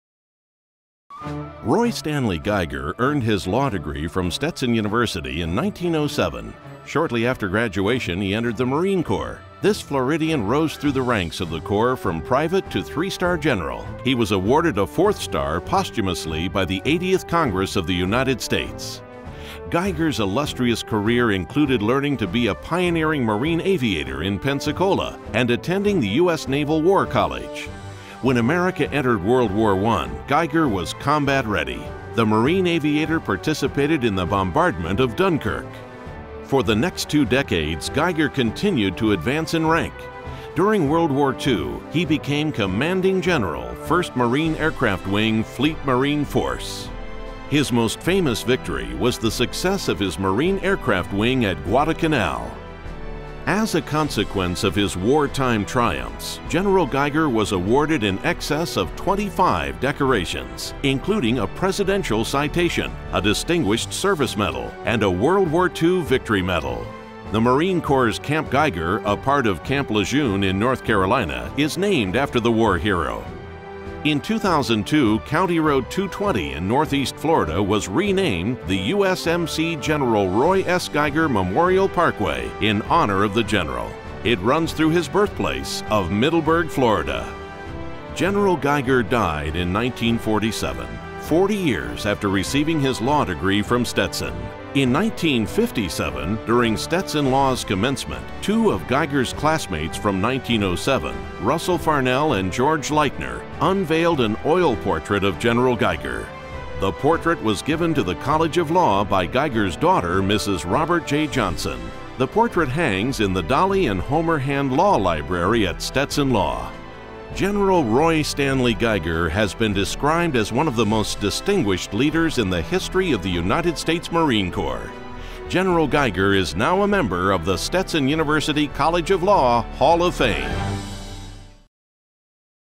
Stetson Law Hall of Fame 2011 - General Roy Stanley Geiger
General Roy Stanley Geiger '07 was inducted into Stetson Law's Hall of Fame on Oct. 29, 2011. Geiger earned his law degree from Stetson Law in 1907. During World War II, he became Commanding General of the First Marine Aircraft Wing, Fleet Marine Force. His most famous victory was the success of his Marine Aircraft Wing at Guadalcanal. General Geiger was awarded in excess of 25 decorations, including a Presidential Citation for the battle of Guadalcanal, a Distinguished Service Medal, and a World War II Victory Medal. Camp Geiger at Camp Lejeune in North Carolina is named after the war hero. Geiger was promoted to four-star rank posthumously by the 80th Congress in 1947.